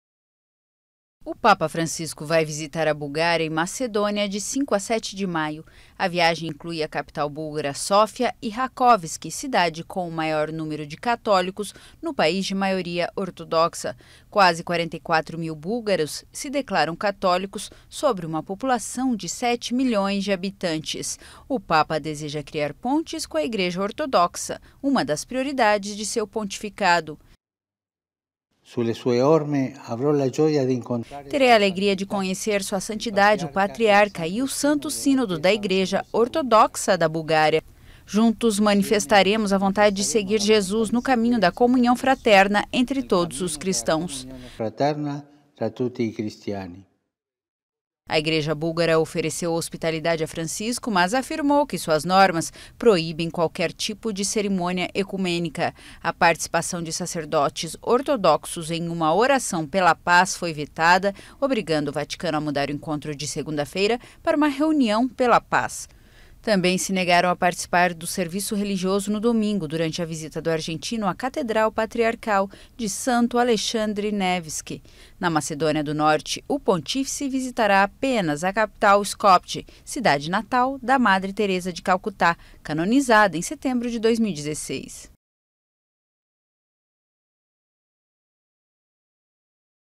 Papa viaja para diálogo com os ortodoxos
O papa Francisco inicia no domingo sua quarta viagem do ano, uma visita à Bulgária e Macedônia, para confirmar sua vontade de diálogo com os ortodoxos e mostrar sua proximidade da minoria católica.